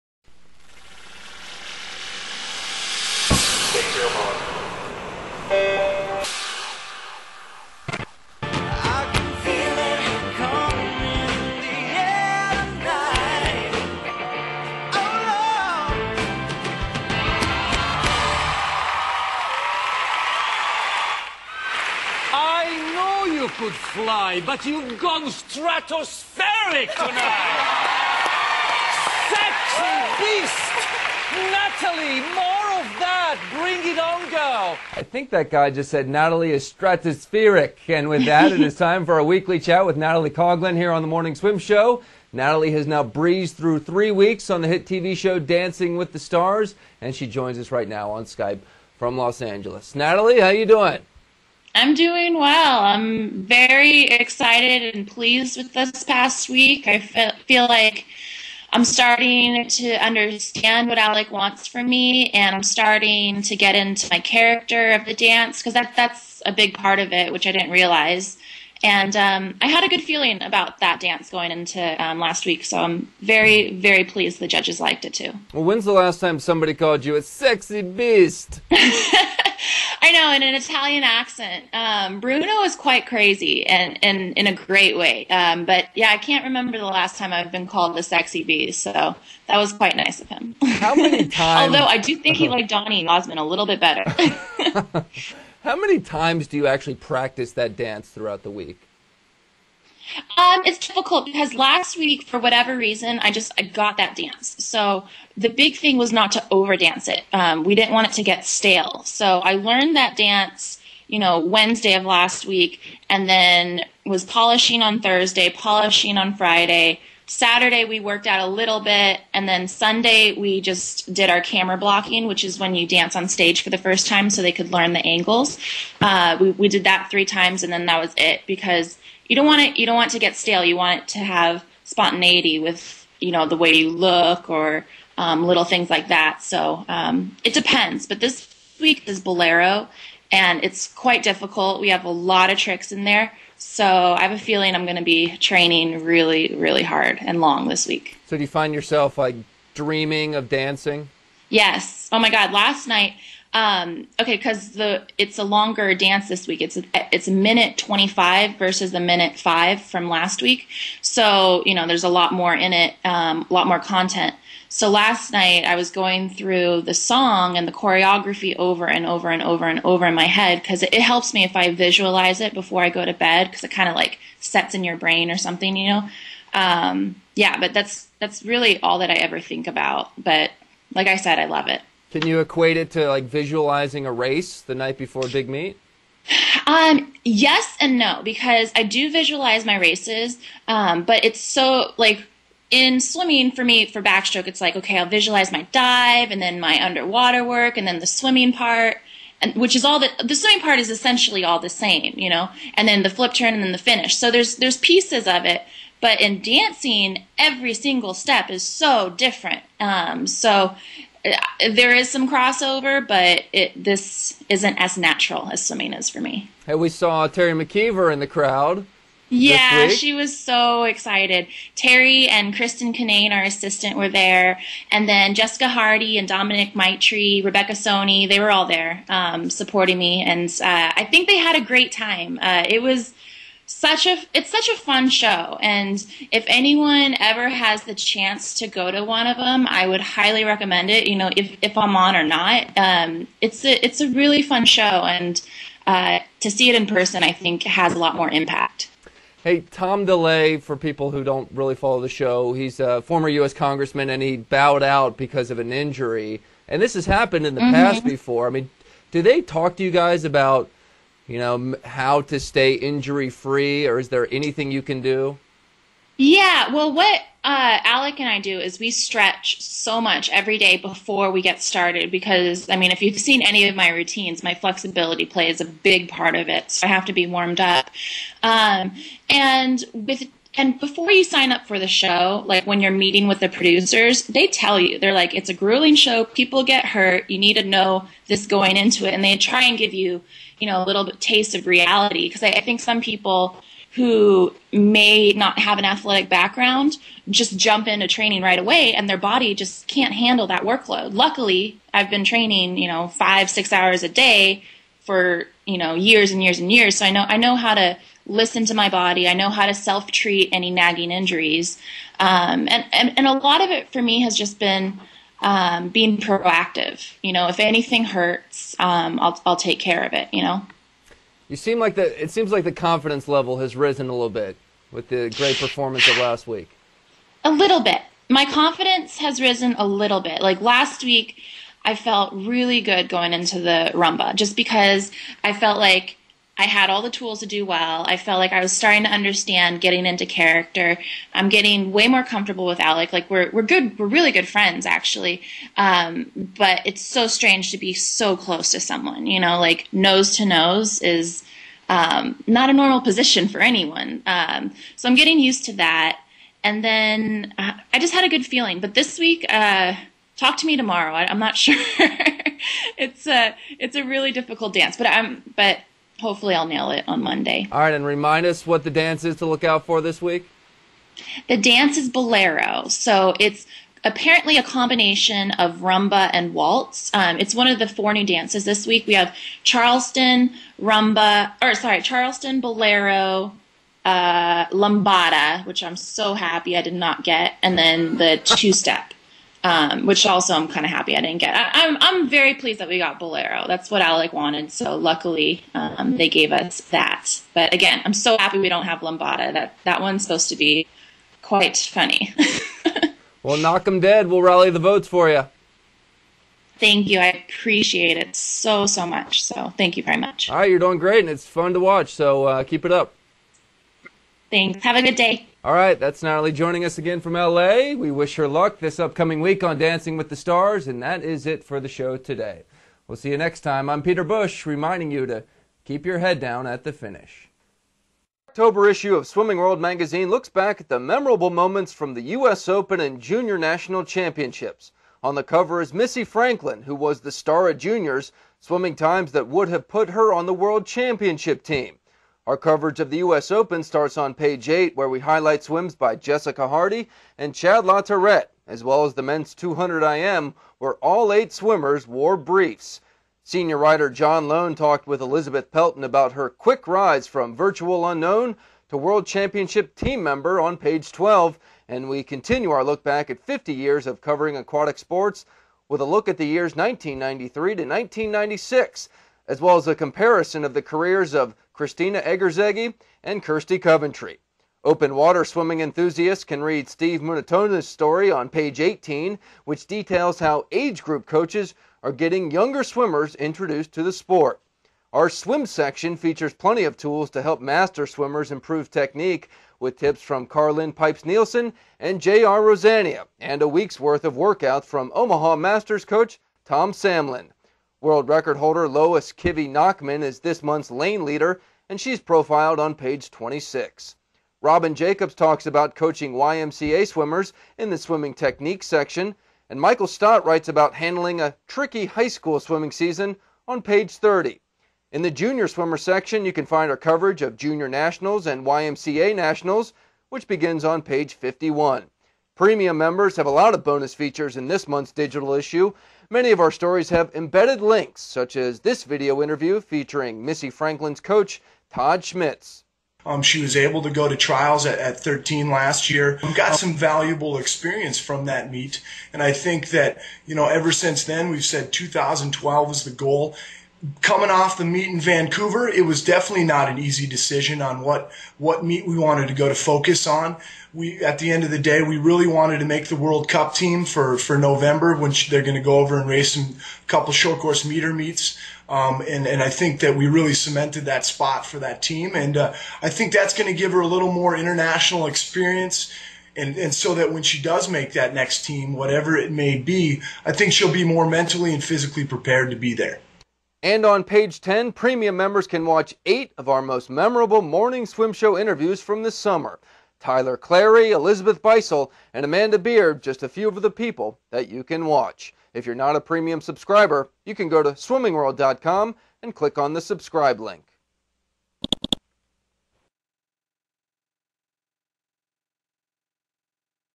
MSS: Natalie Coughlin
Oct. 9, 2009: The Olympic champion talks about dancing the rumba in front of her longtime swimming coach, Teri McKeever, why she&apos;s glad she&apos;s dancing the bolero in next week&apos;s episode of &quot;Dancing With the Stars&quot; and how she and partner Alec Mazo are staying injury-free each week.